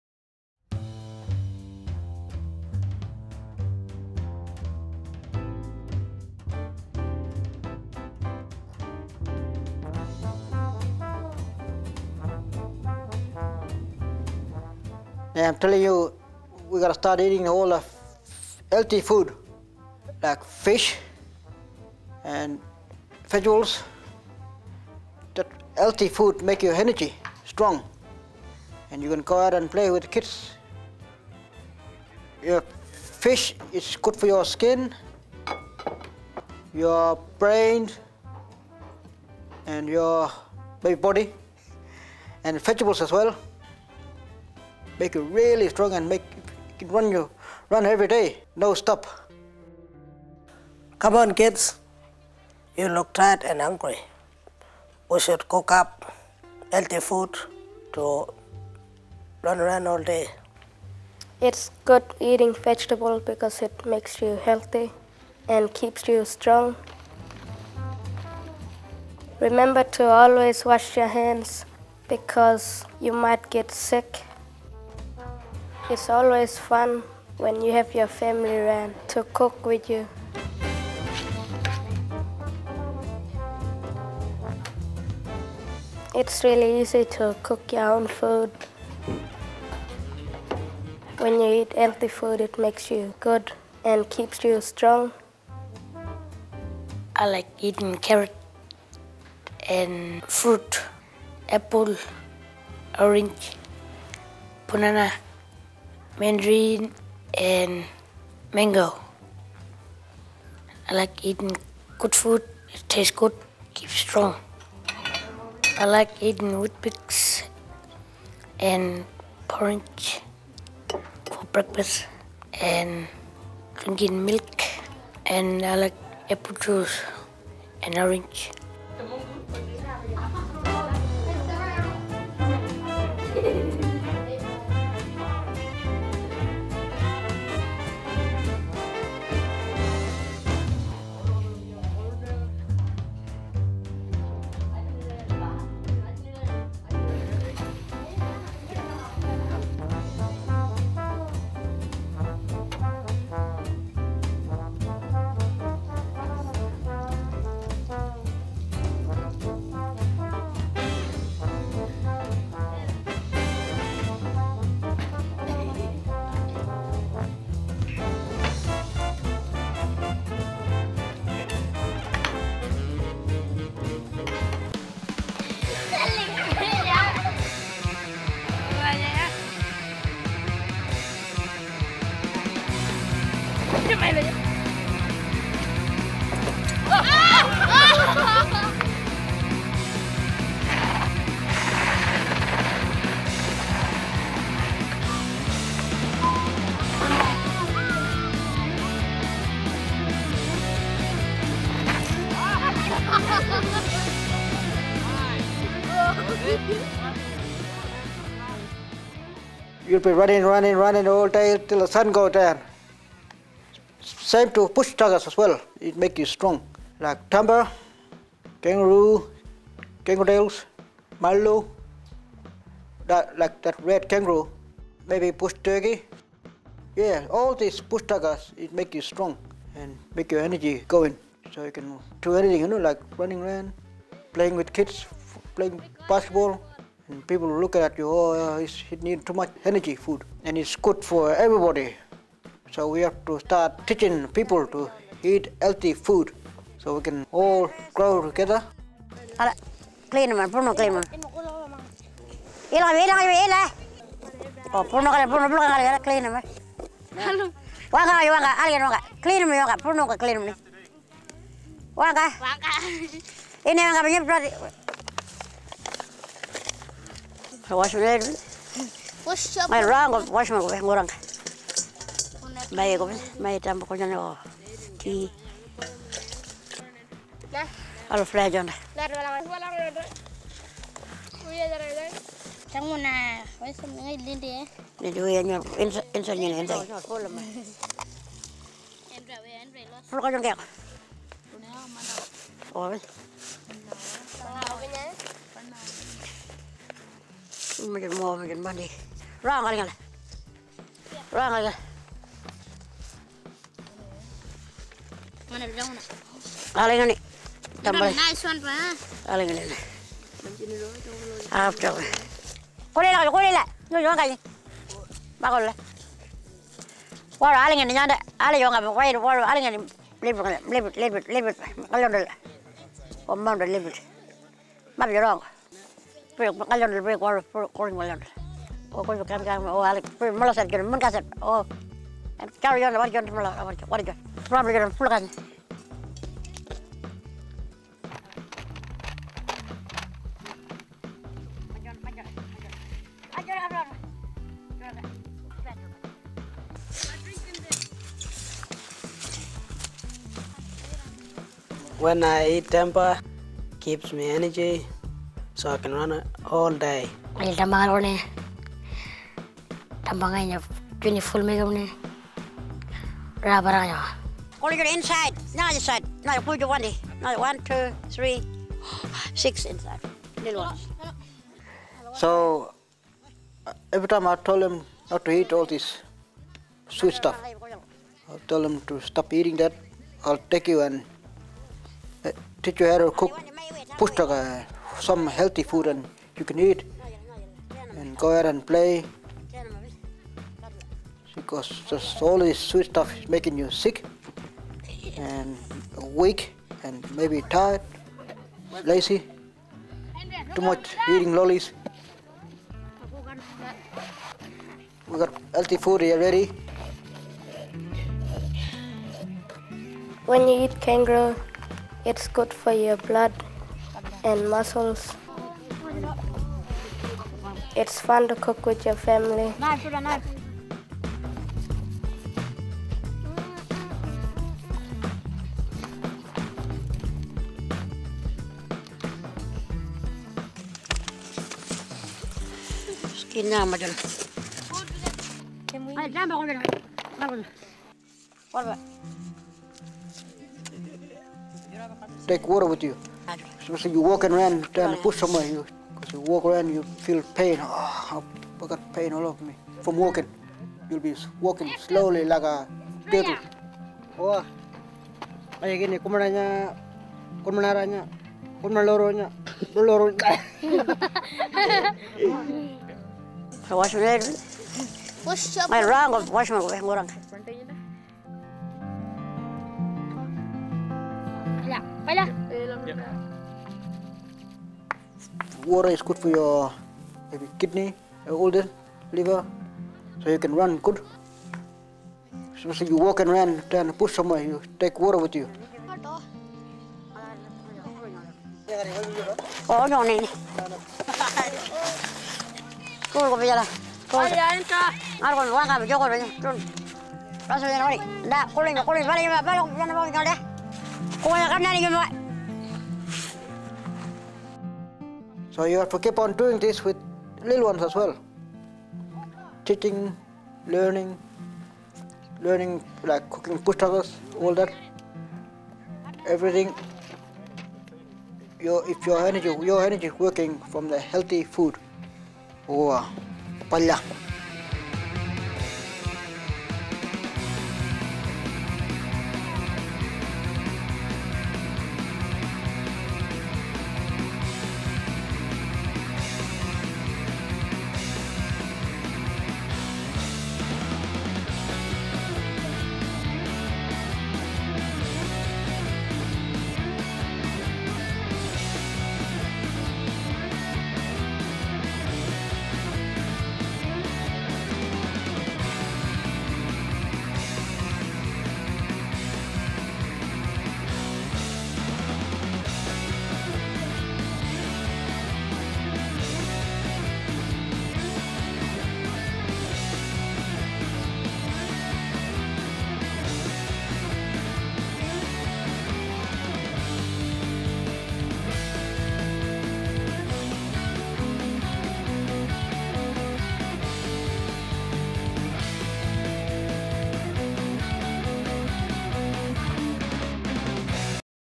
Yalata - Good food Strong body Good mind
2009 Yalata
The Hero Project worked with over 30 people from the Yalata community and Tullawon Health Service to produce a short film. During the 4-day workshop the young people brainstormed, shot and edited their film to promote healthy eating and active lifestyle for Indigenous youth.

Mai Palya - Anangu Kunpu - Kata Palya:
Cmon kids, you look tired! Lets go and cook something healthy to eat, itll give you energy to run and play all day!
Fish is good for the brain, vegetables keep you healthy and bush tucker like kangaroo tails keeps you strong!

The Hero Project
The award-winning Hero Project youth empowerment program offers hands-on digital video training, resources, workshops, a youth media festival and interactive website.